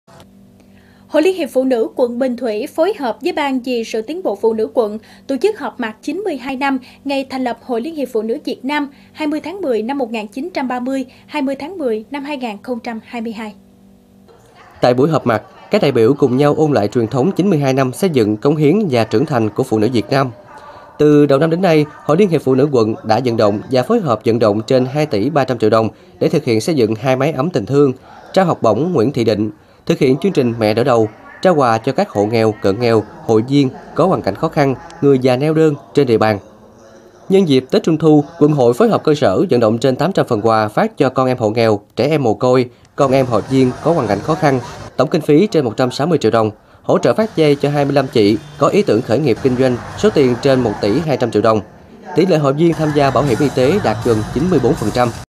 Bình Thủy họp mặt kỷ niệm 92 năm ngày thành lập hội LHPN Việt Nam| Cần Thơ TV
Hội Liên hiệp phụ nữ quận Bình Thủy phối hợp với Ban vì sự tiến bộ phụ nữ quận tổ chức họp mặt kỷ niệm 92 năm ngày thành lập Hội Liên hiệp phụ nữ Việt Nam (20/10/1930-20/10/2022).